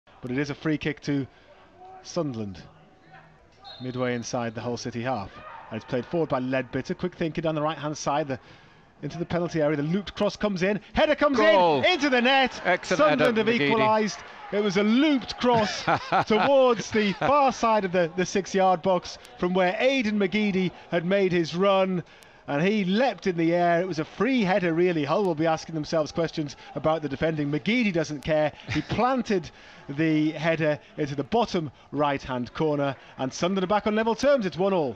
MATCHDAY MOMENT: McGeady vs Hull City
See Aiden McGeady's equaliser against Hull City from an alternative angle